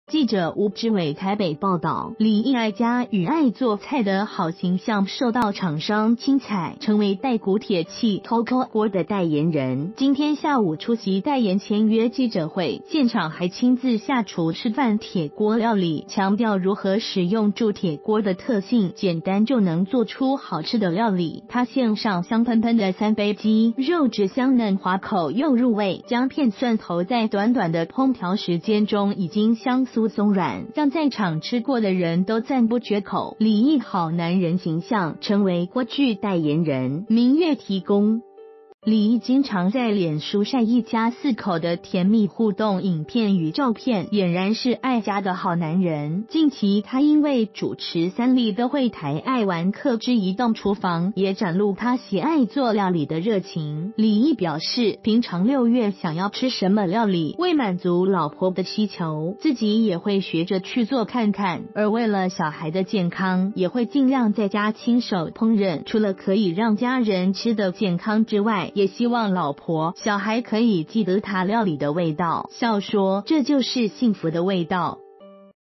李易樂當煮夫 要讓家人記住「幸福的味道」
〔記者吳志偉／台北報導〕李易愛家與愛做菜的好形象受到廠商青睬，成為大古鐵器COCO鍋的代言人，今天下午出席代言簽約記者會，現場還親自下廚示範鐵鍋料理，強調如何使用鑄鐵鍋的特性，簡單就能做出好吃的料理。他獻上香噴噴的三杯雞，肉質香嫩滑口又入味，薑片蒜頭在短短的烹調時間中已經香酥鬆軟，讓在場吃過的人都讚不絕口。李易好男人形象，成為鍋具代言人。（明悅提供）

發表評論評論如下。